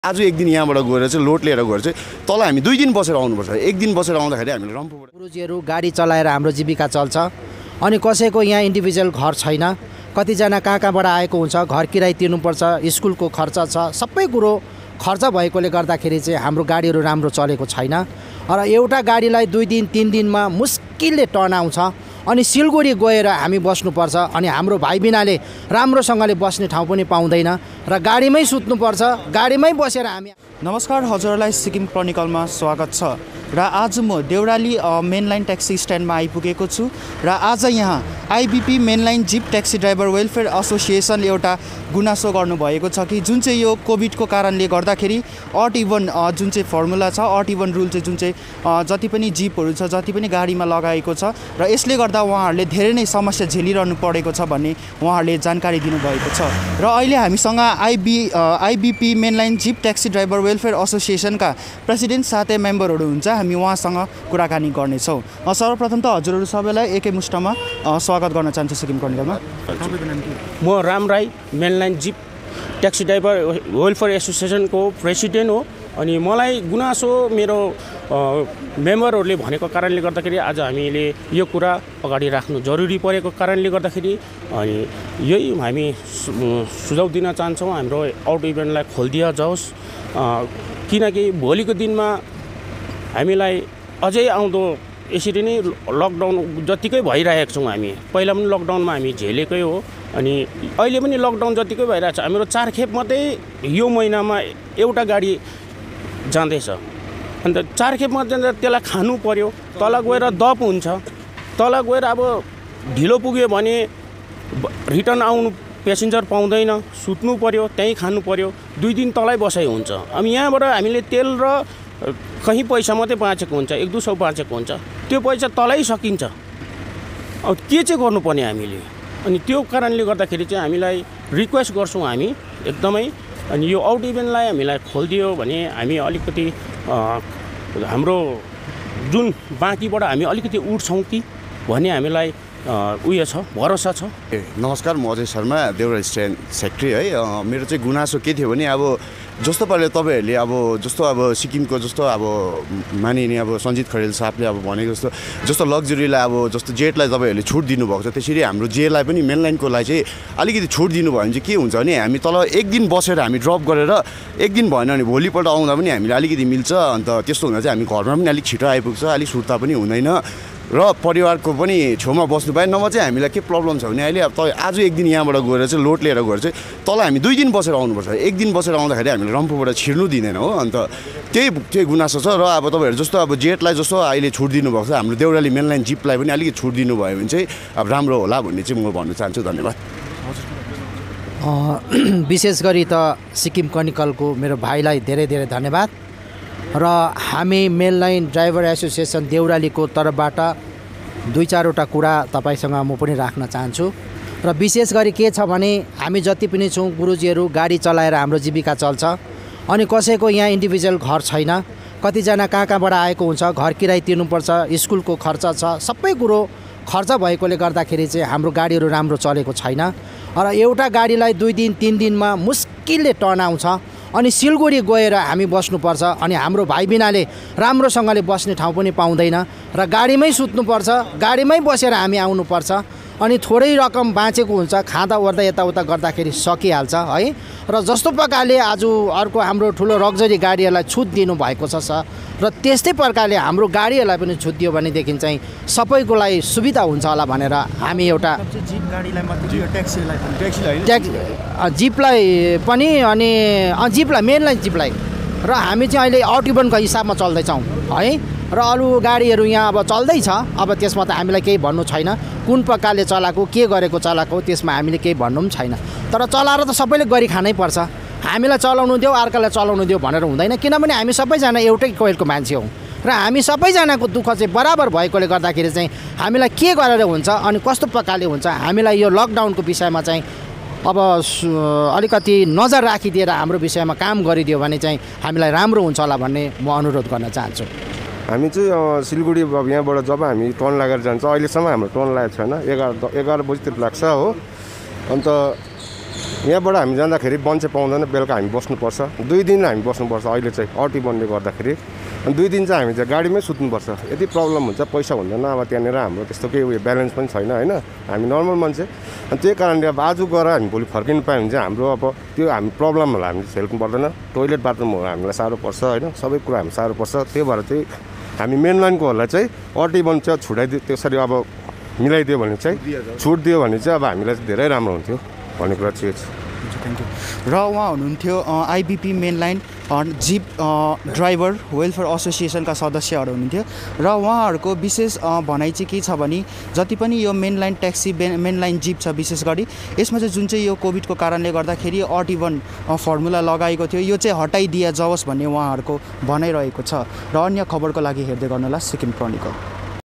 Indira Bye-Pass (IBP) mainline Jeep Taxi Driver welfare Association demands the removal of Odd Even
Indira Bye-Pass (IBP) mainline Jeep Taxi Driver welfare Association demands the removal of Odd Even restrictions.

Report By Suben Pradhan